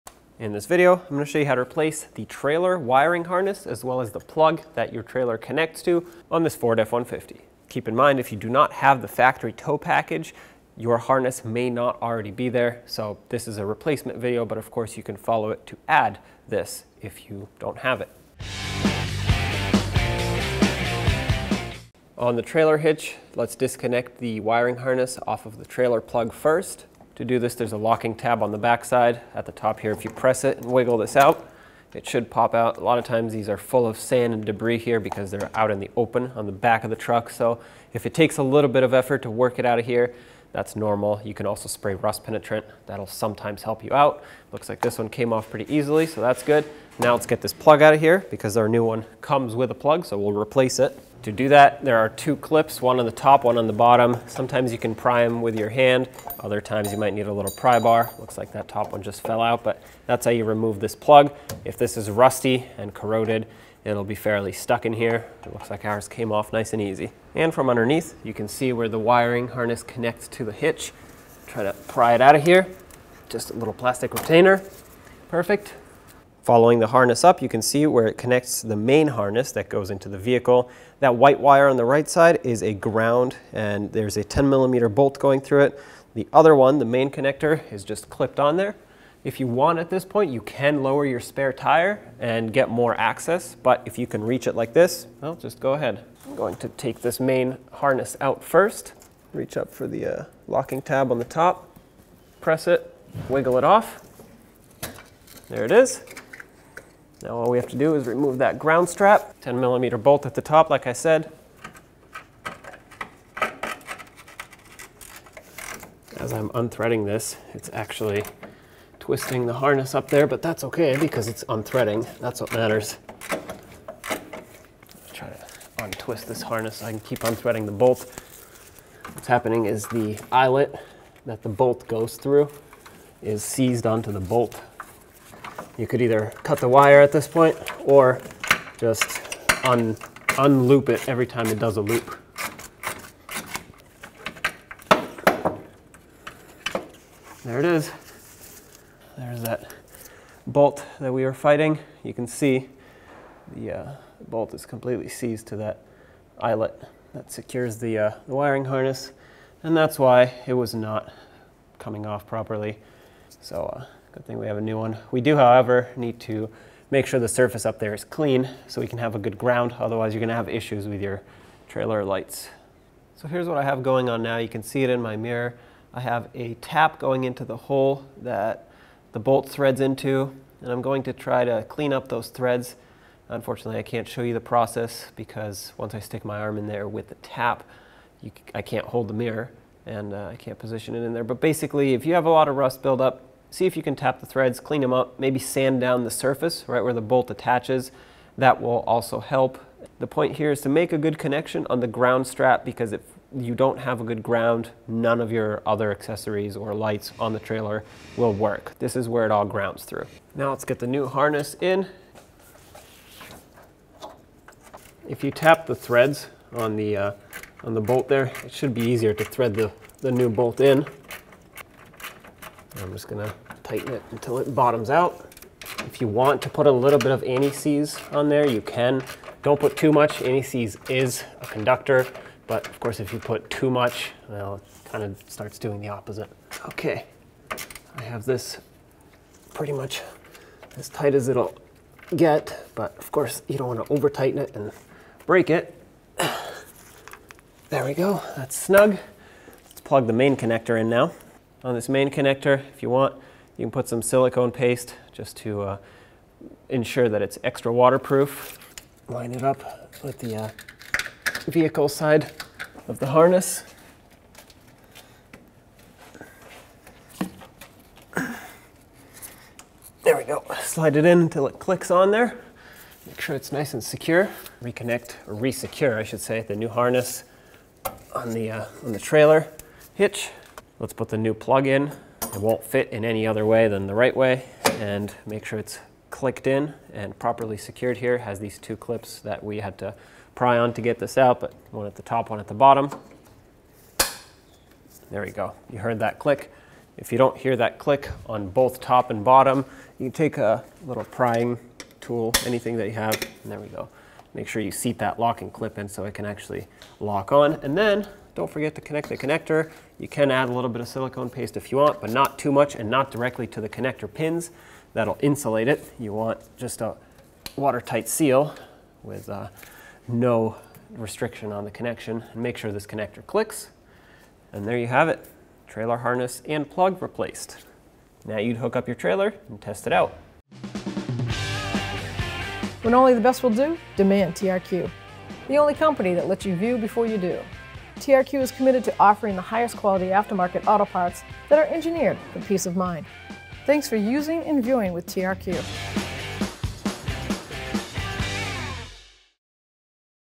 How to Replace Trailer Wiring Harness 2009-2014 Ford F-150
This video shows you how to install a new trailer wiring harness from TRQ on your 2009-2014 Ford F-150.

This process should be similar on the following vehicles:
2009 Ford F-150
2010 Ford F-150
2011 Ford F-150
2012 Ford F-150
2013 Ford F-150
2014 Ford F-150